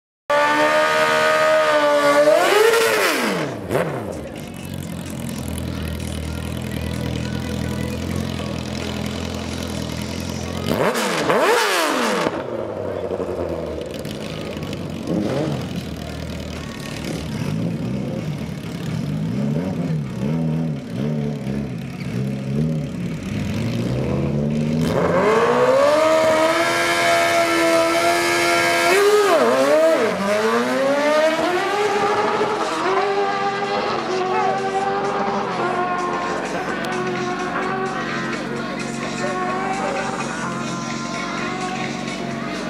November 2013 QTM Day 4 Run 43
13th December 2013 - Day 4 of the Final QTM event for 2013 hosted by the MDRA at Hal Far Raceway...

Finally the sun came out and we could finish the November QTM.